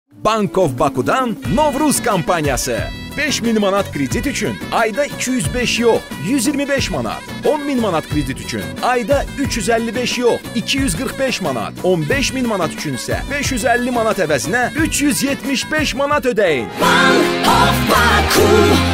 Bank of Baku-dan Novruz kampaniyası!
Novruz gəlir, yaz gəlir, Bank of Baku-ya endirim gəlir!😍

İndi Bank of Baku-dan 30.000 manatadək nağd pul kreditini min.12.5% ilə əldə edin,

🌸5000 AZN kredit üçün ayda 205 yox, 125 AZN;
🌸10.000 AZN kredit üçün ayda 355 yox, 245 AZN;
🌸15.000 AZN üçün isə 550 AZN əvəzinə 375 AZN ödəyin.